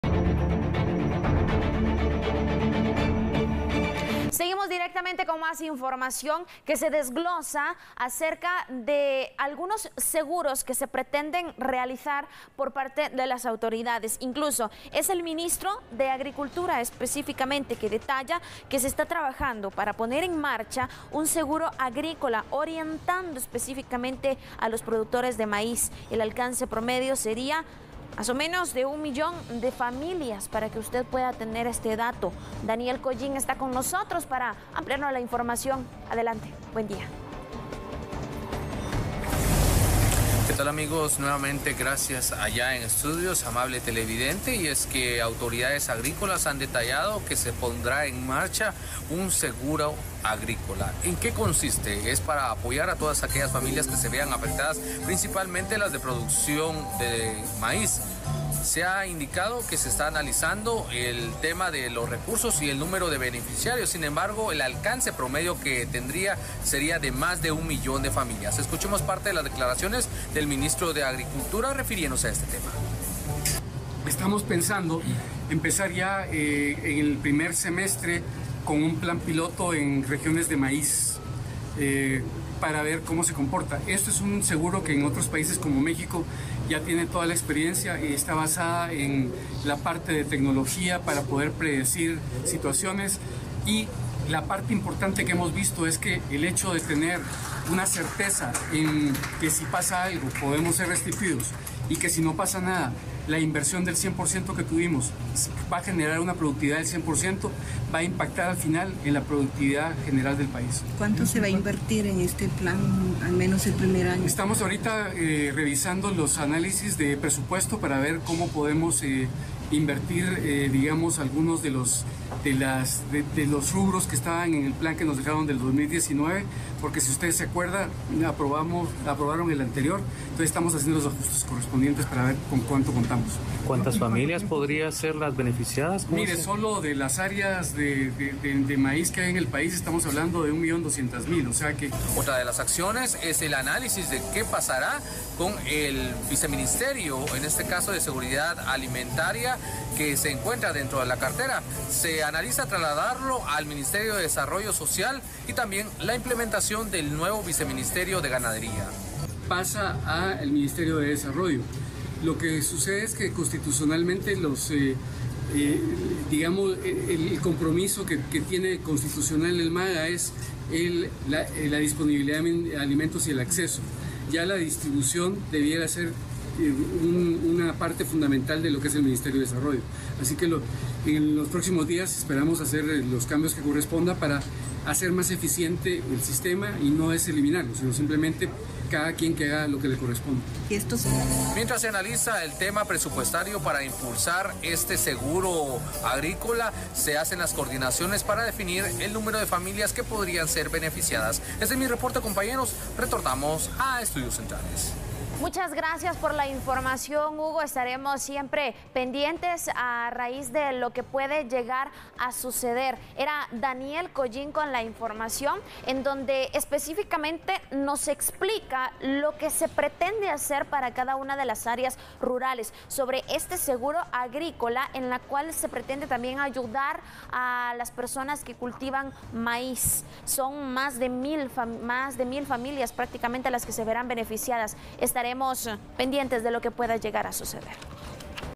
MAGA trabaja para poner en marcha seguro agrícola
Analizan recursos y número de beneficiados

Autoridades del MAGA trabajan para dar marcha a un plan de seguro agrícola que busca aumentar la productividad del país. Visita nuestro sitio